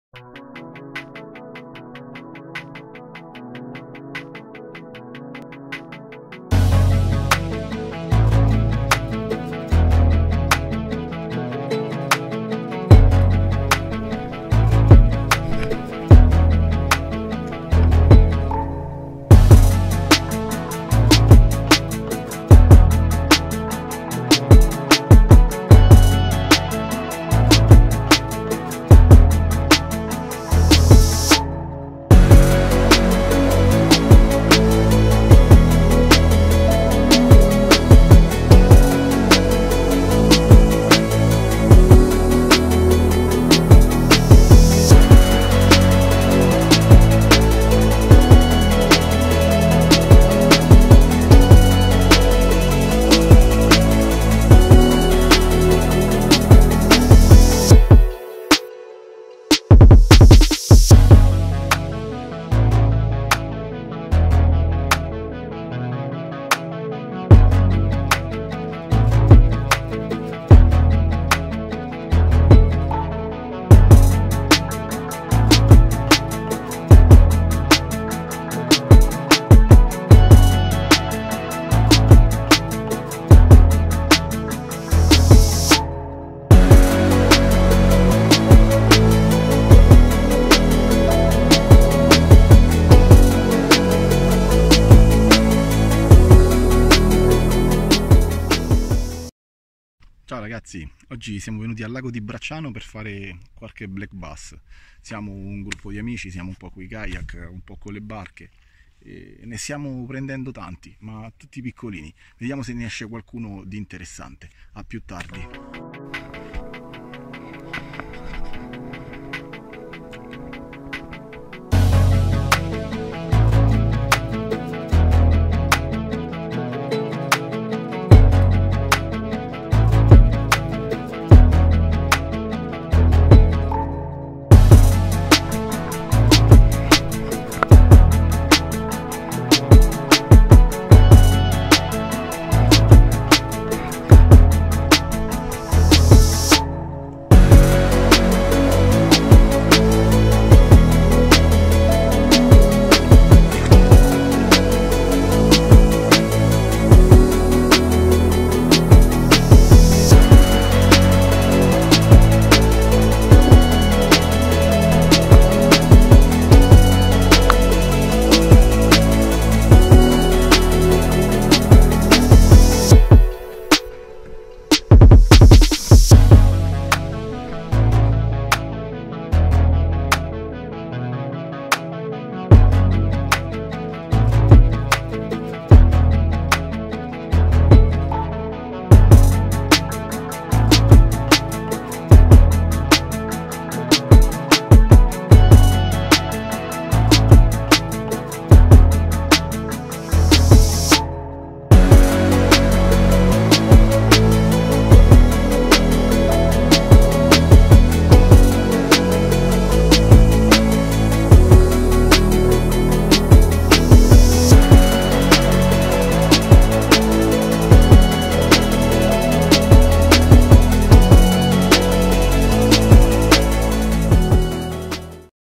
pesca spinning lago di bracciano kayak fishing "ep. 07 serie 2022"
Anche se luglio non è dei mesi migliori per affrontare il lago di Bracciano abbiamo deciso di organizzare una pescata tutti insieme prima di iniziare le nostre vacanze estive, sono usciti tantissimo bass ma tutti di piccola taglia, ci siamo divertiti comunque così ho deciso di montare questo piccolo video a ricordo della giornata passata insieme!